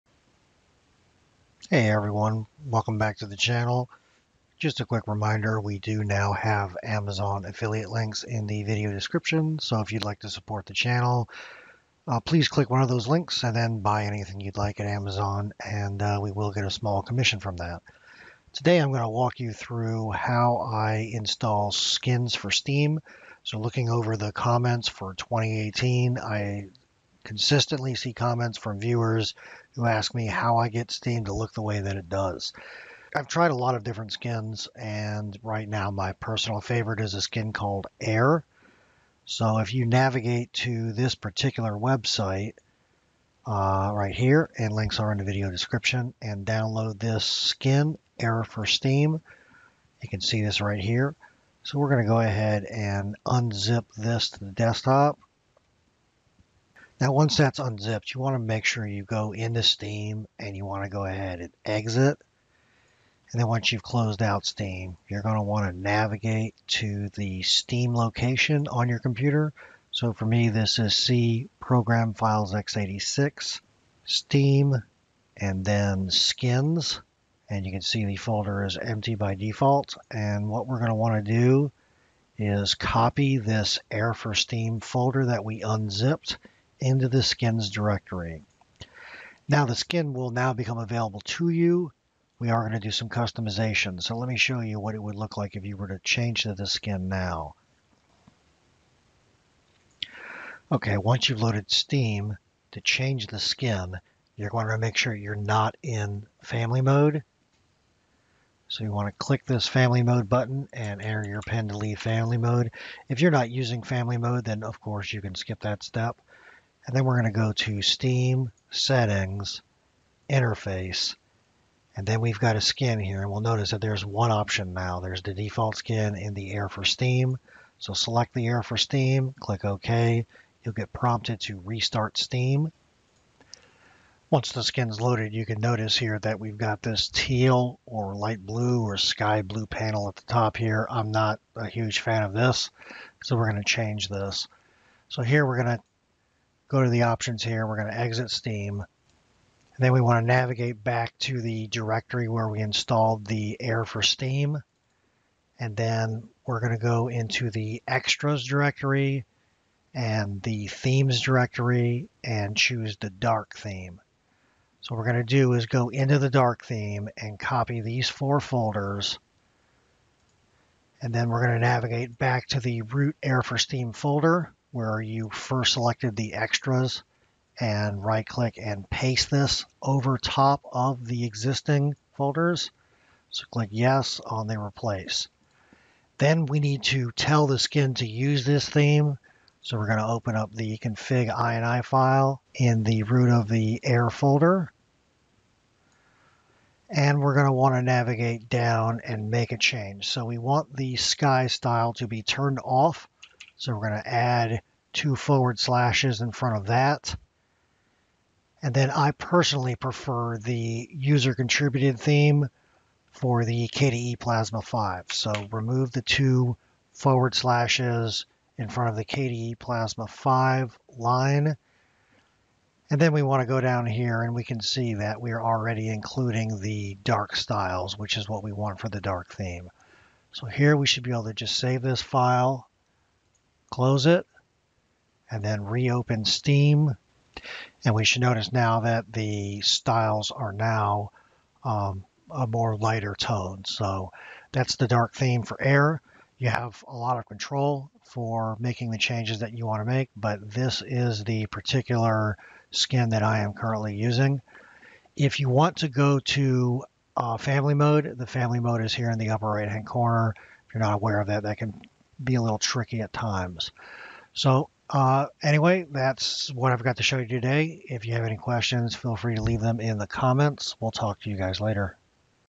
Setup the Most Beautiful Steam Client Possible - Air for Steam Skin
In this guide I walkthrough how to install and configure the Air for Steam skin, which I personally think is the most visually appealing skin currently available for the Steam gaming client on PC.

Fallout: New Vegas Ultimate Edition (PC)